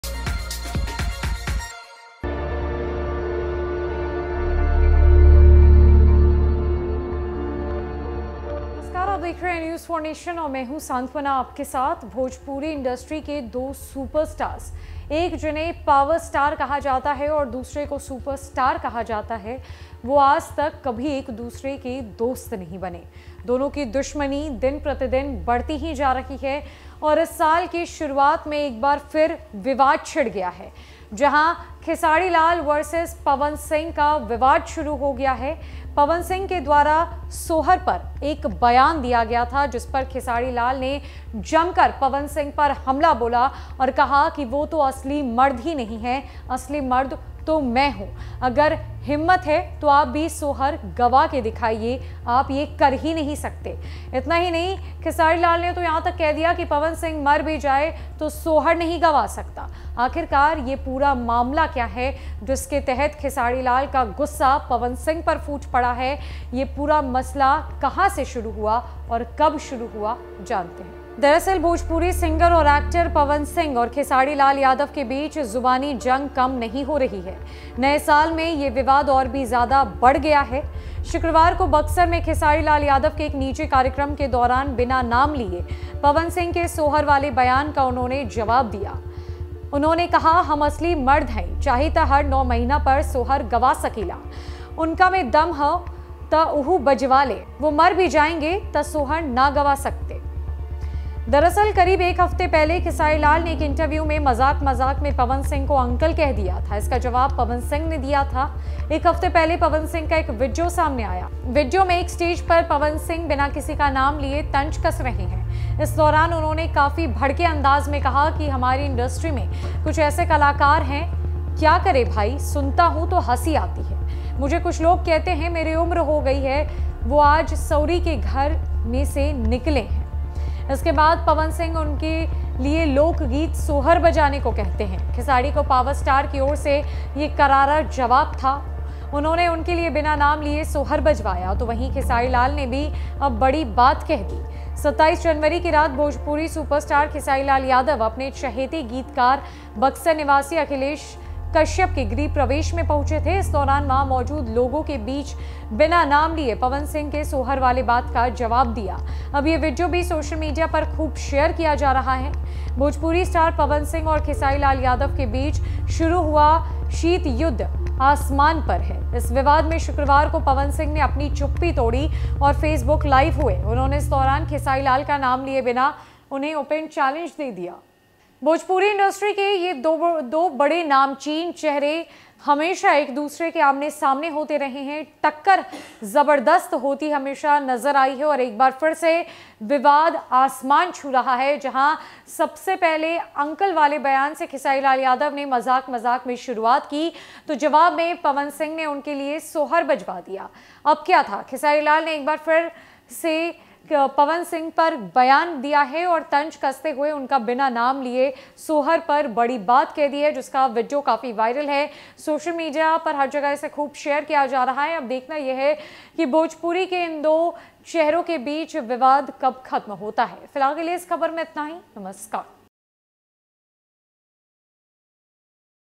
Pawan Singh और Khesari Lal के बीच हो गया ज़बरदस्त विवाद, अब "सोहर" पर खेसारी का आया बड़ा बयान, सुनिए
Pawan Singh और Khesari Lal के बीच हो गया ज़बरदस्त विवाद, अब "सोहर" पर खेसारी का आया बड़ा बयान, सुनिए | News4Nation